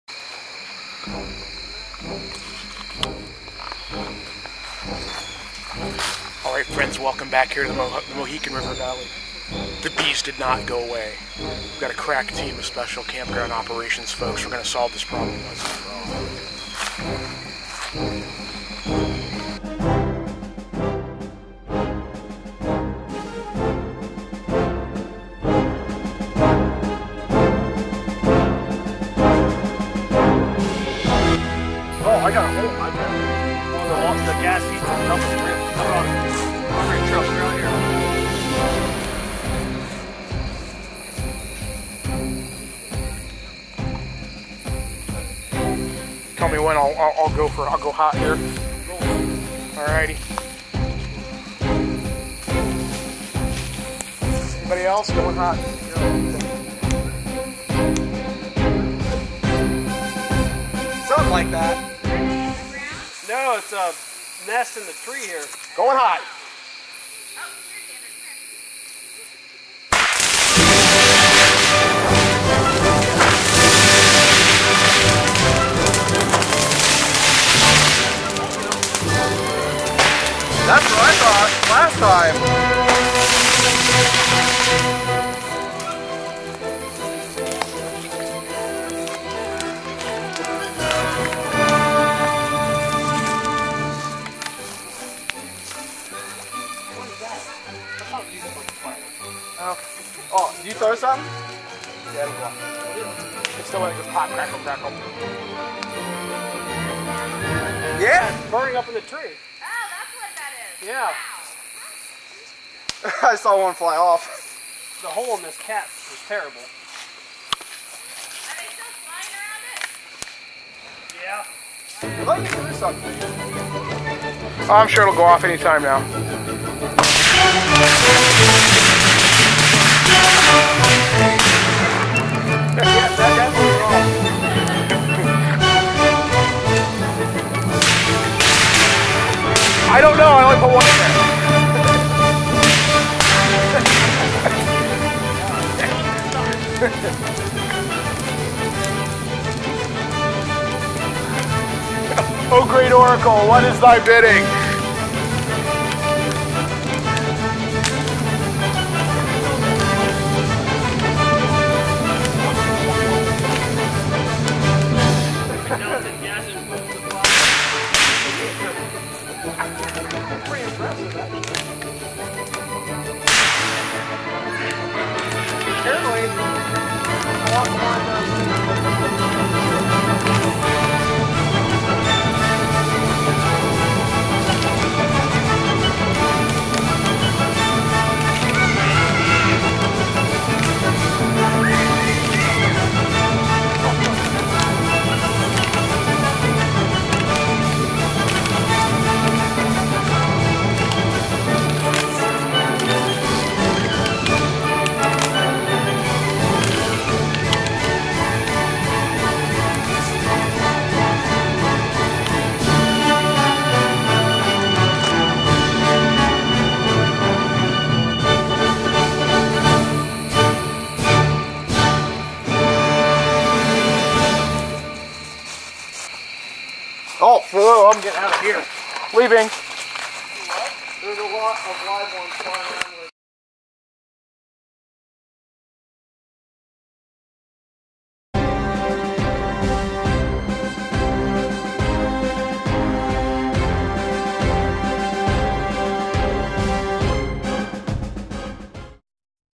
Hate filled Bald Face Hornets........ The REVENGE!!!!!!! Third time is the charm.
After the bees met the fires of Hades. they came back the very next day. They just wouldn't stay away...… After assembling a crack team of special camp operations quick reaction force members, we issue a devastating blow and prove problem solving is a team effort.